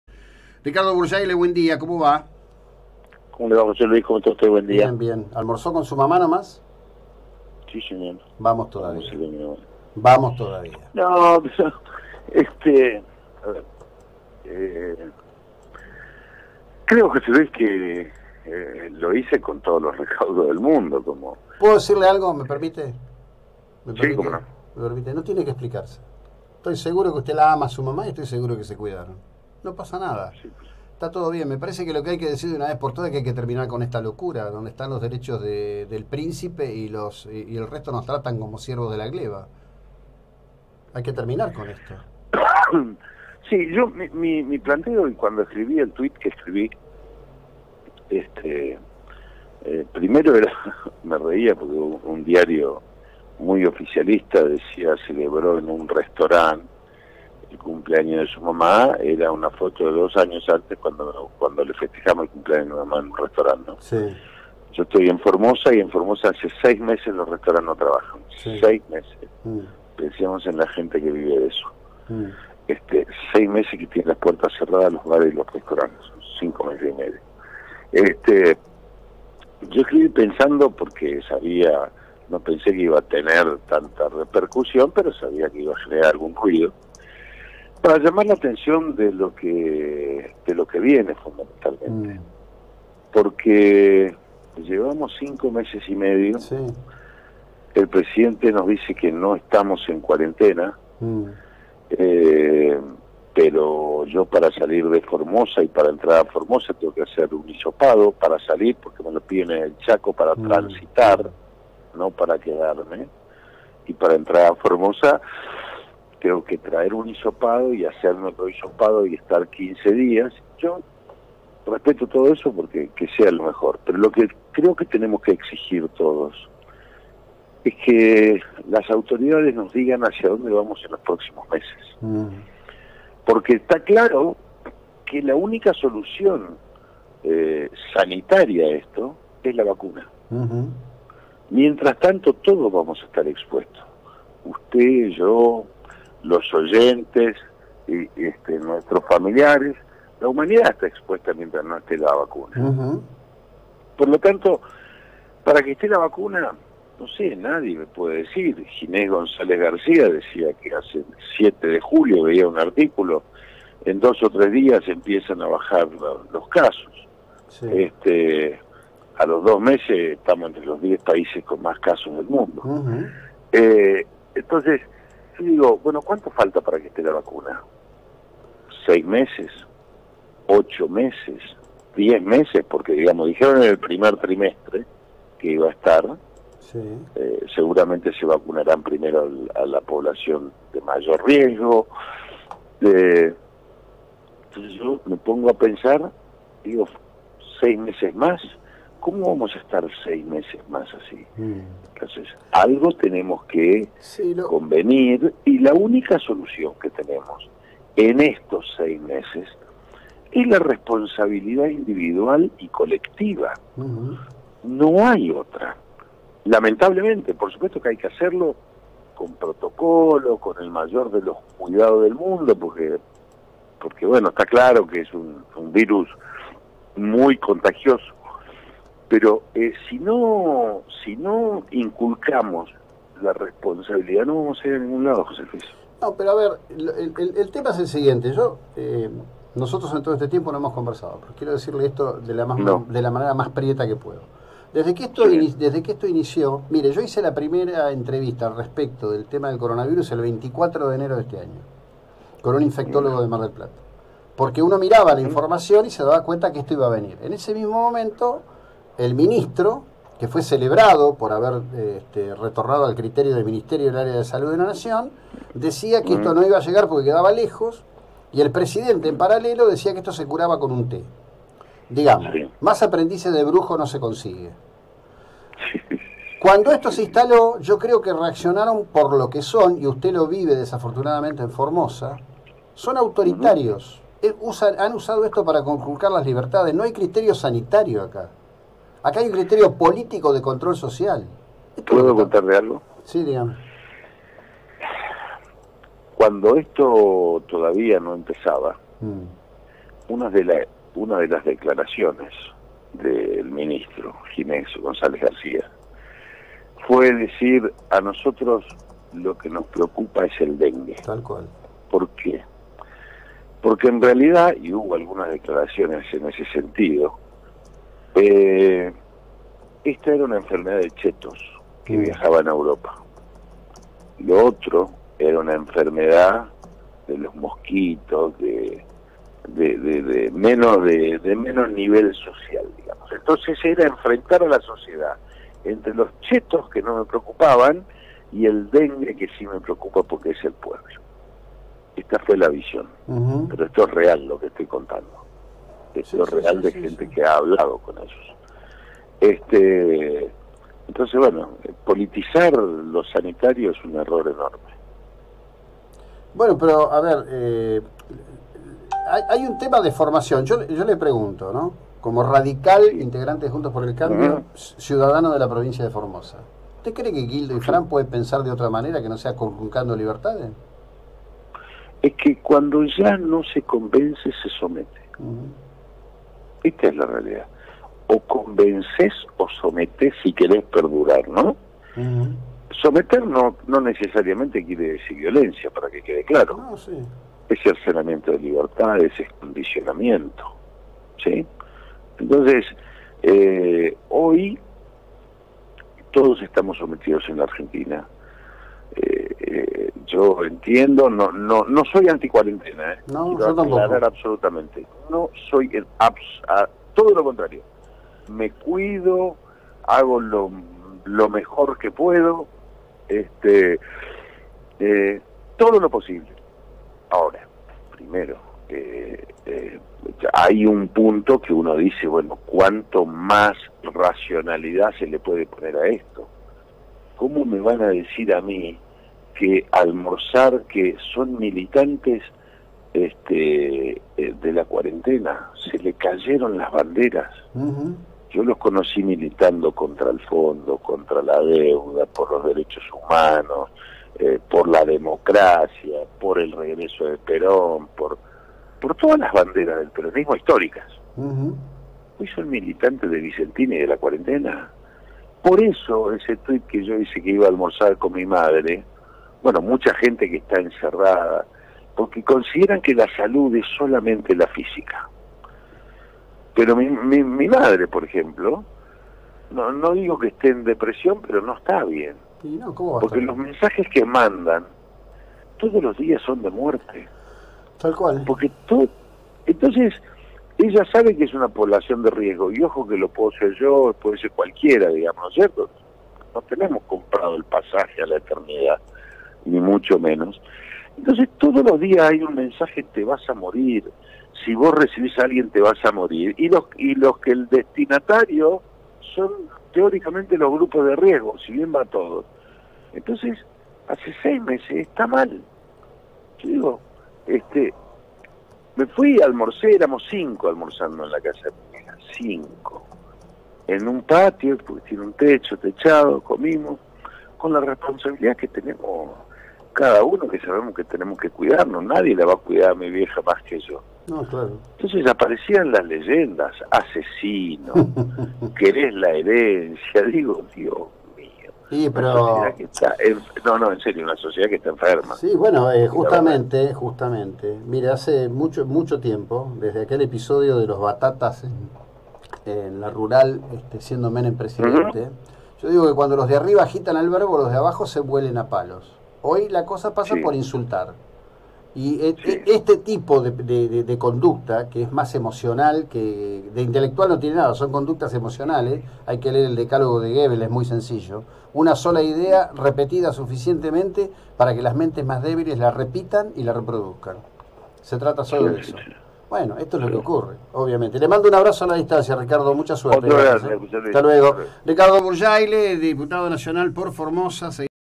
RICARDO BURYAILE EN N&P 01-09-2020
El Diputado Nacional Ricardo Buryaile se refirió al almuerzo que compartió con su madre y criticó la forma de manejo de la cuarentena en el país.